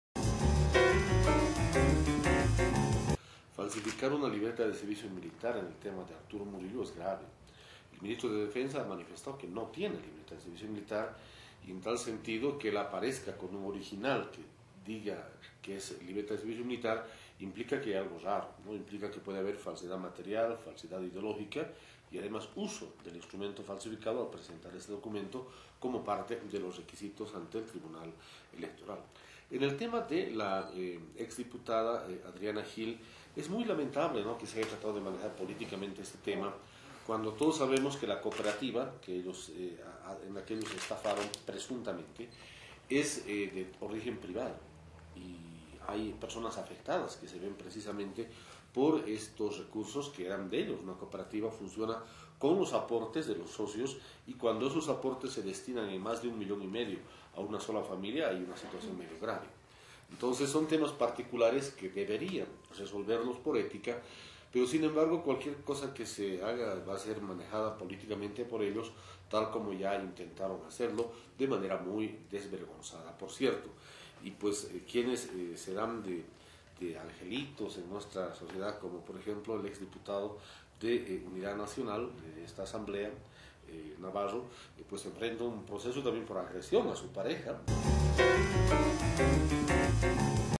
Diputado Marcelo Elío - Candidatos de la oposición
El presidente de la Cámara de Diputados, Marcelo Elío, señaló que candidatos de la oposición están acusados de delitos.